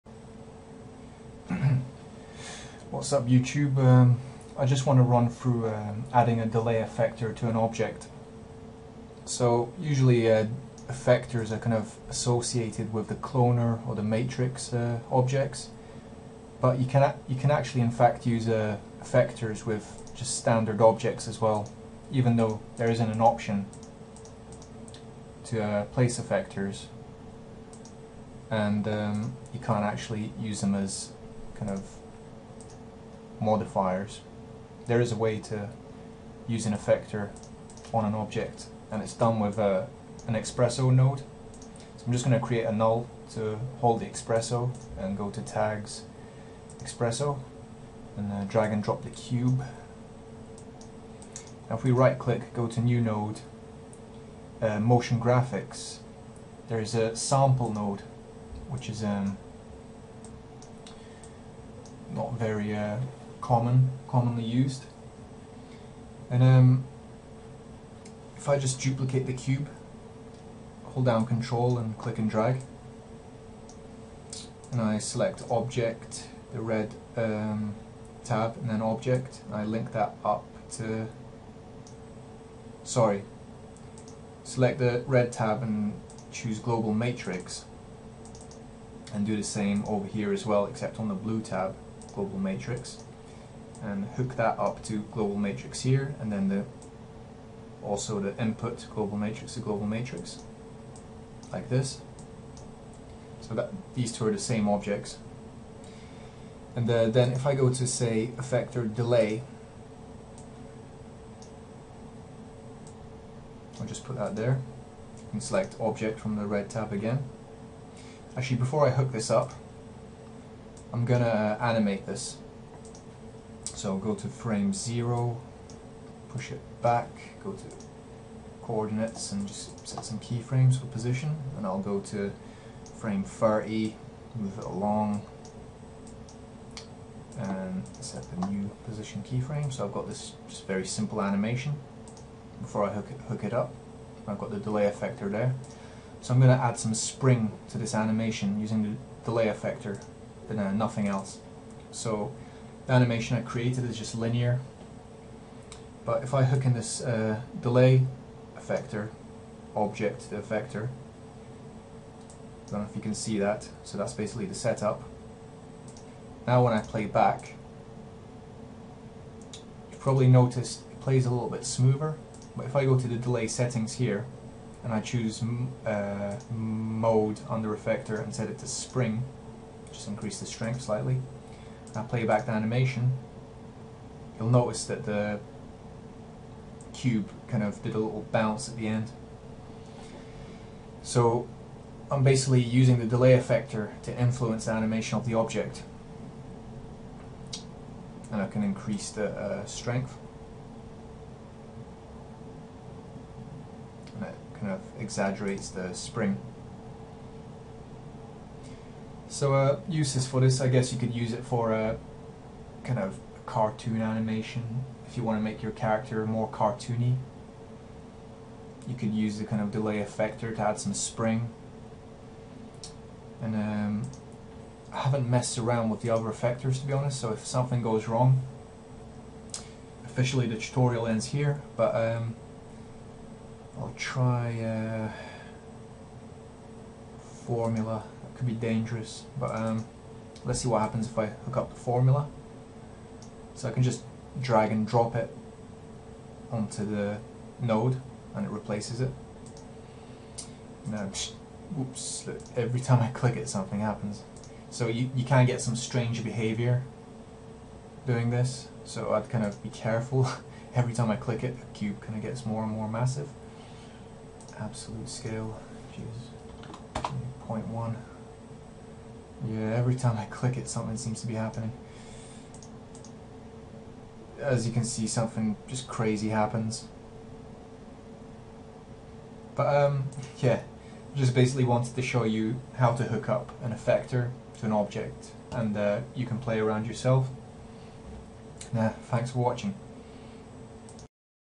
Cinema4D Tutorial: Use Effectors With Objects (Beginner)
If you found this tutorial useful, you can Use Effectors With Objects, using the XPresso Sample method.